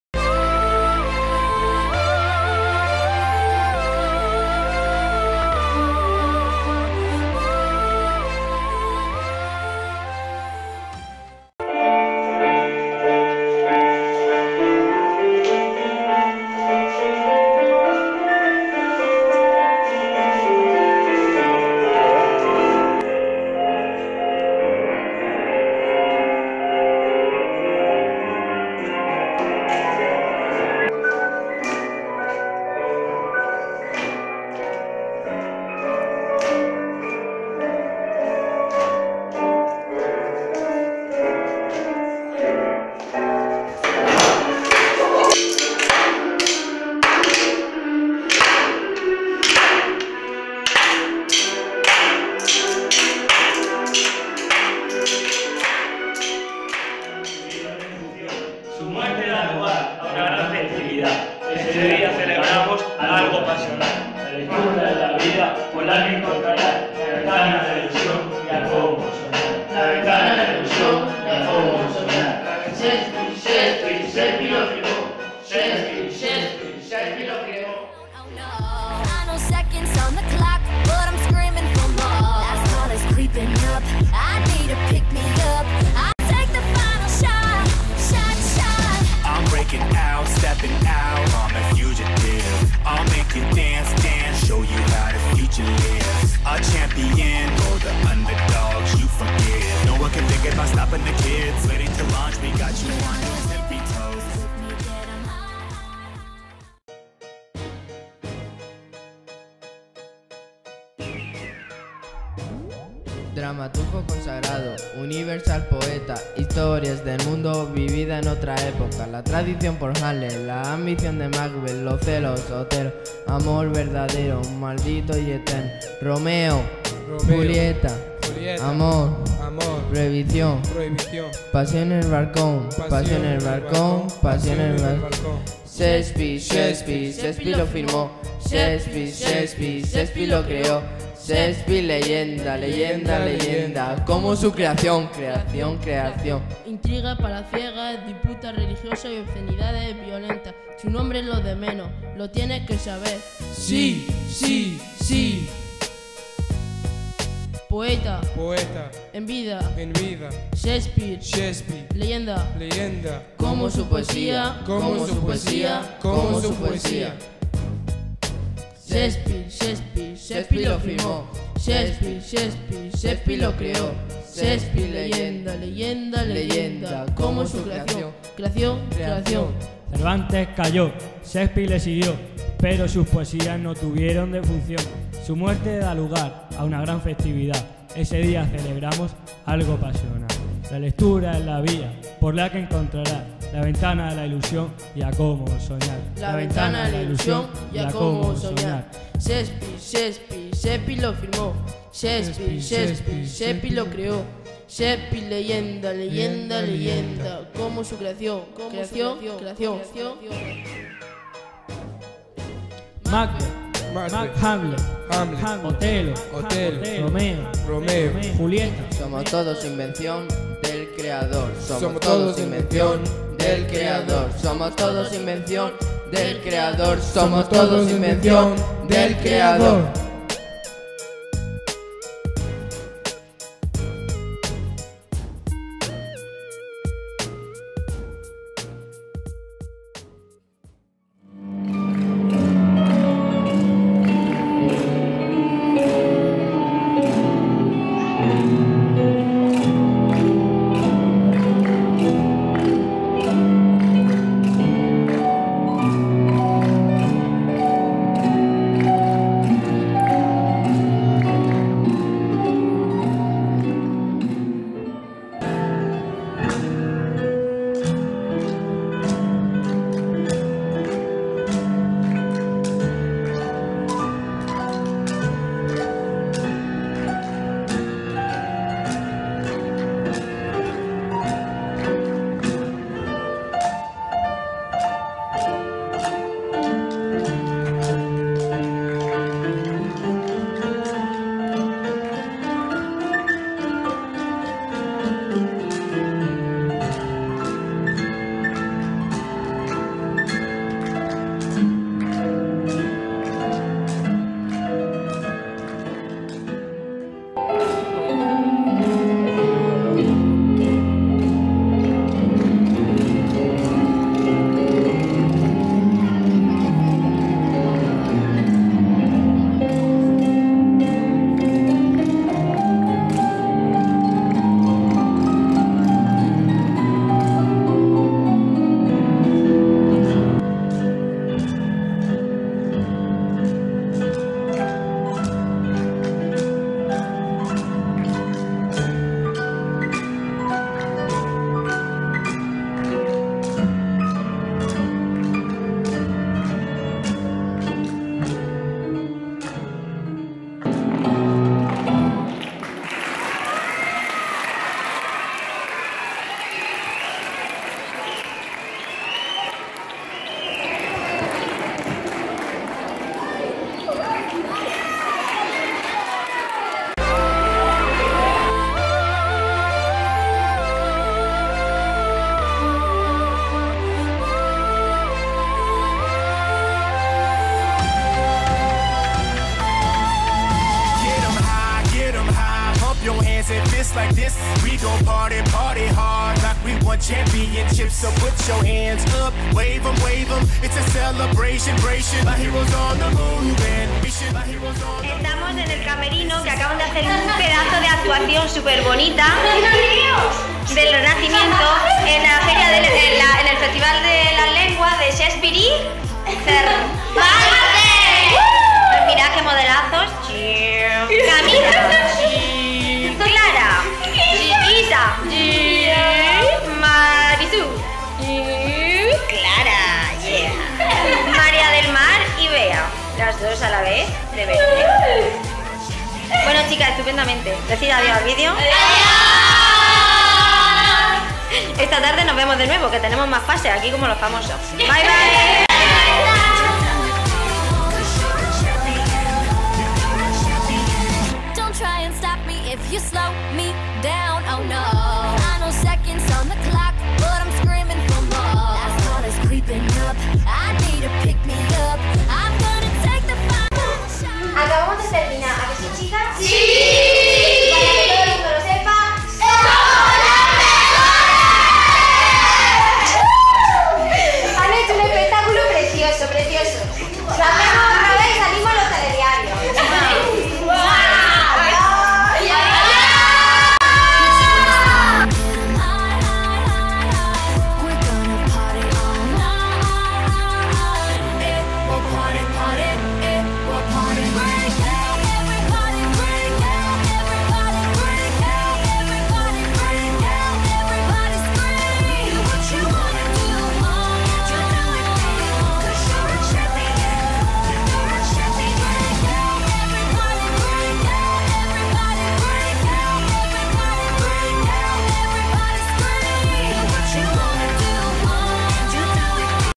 Festival de las Lenguas Modernas. CEIP Profesor Tierno Galván
Vídeo resumen de ensayos y actuaciones de los alumnos de 6º EPO,  1º y 2º ESO del  CEIP. Profesor Tierno Galván en el Festival de las Lenguas modernas, en honor a Shakespeare y Cervantes, que organiza anualmente el IES.Cura Valera de Huércal-Overa (Almería).